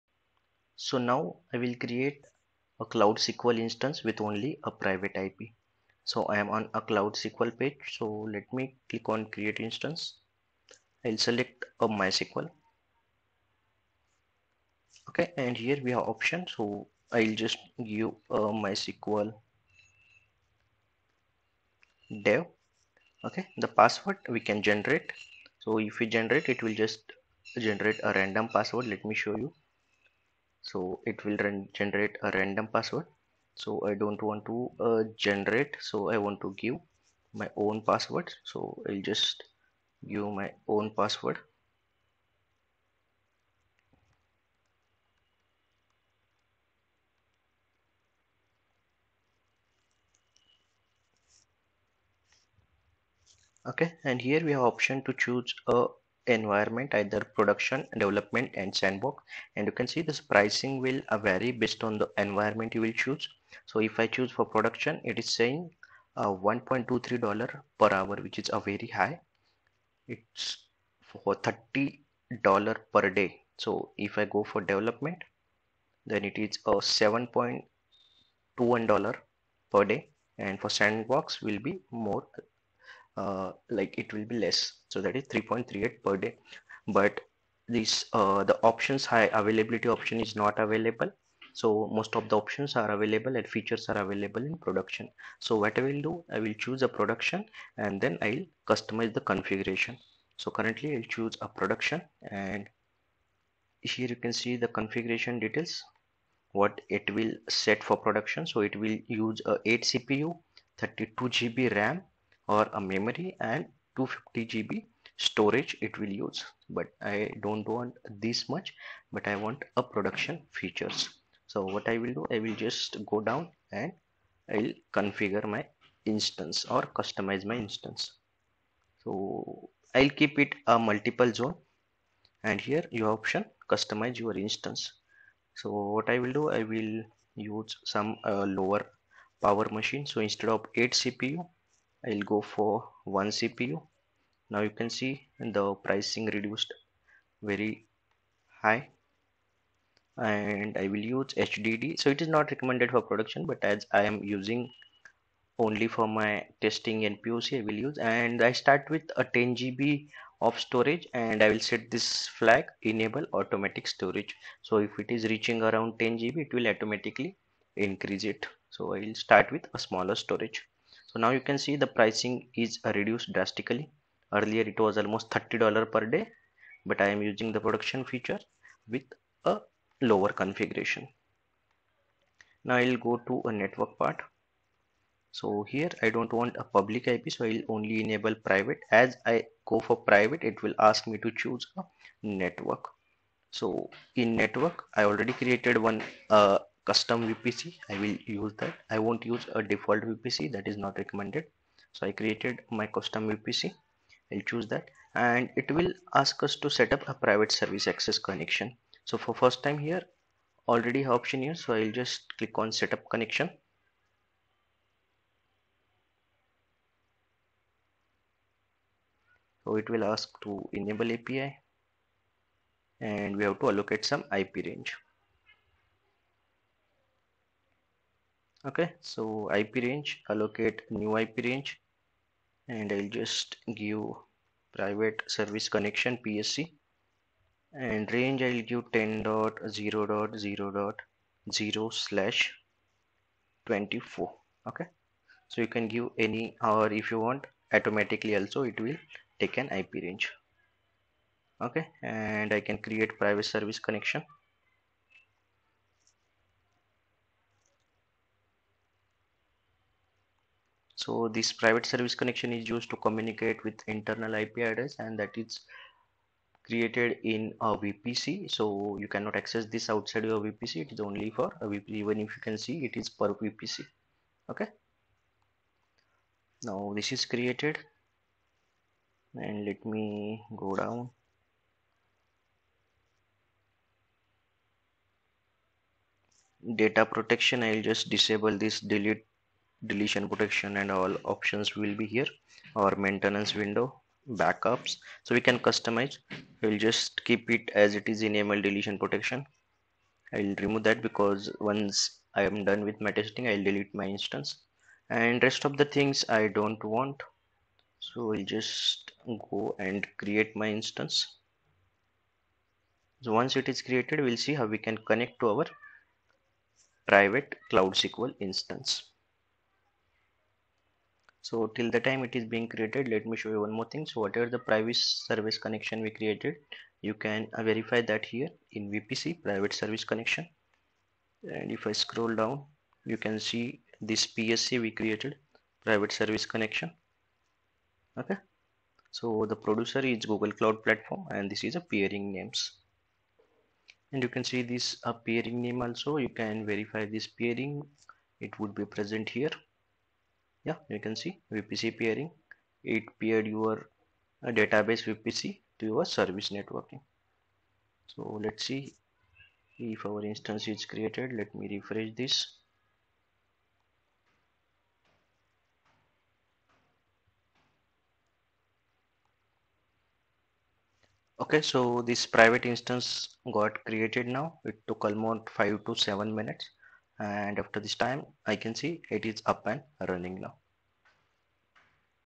Create private cloudSQL Instance
In this video, we dive into Private Cloud SQL, a fully managed database service offered by Google Cloud. You’ll learn how Private Cloud SQL allows organizations to run databases in a secure private environment with no public IP exposure.

We’ll cover:

The concept of private IP in Cloud SQL.
How to set up and configure private networking for Cloud SQL.
Key benefits like enhanced security, reduced network latency, and compliance with strict data privacy requirements.
Supported database engines (MySQL, PostgreSQL, SQL Server).
Use cases for private Cloud SQL, such as internal applications and enterprise-level deployments.
This video is perfect for cloud enthusiasts, developers, and IT professionals looking to understand how to securely deploy databases in Google Cloud using private networking.

Github
Source Code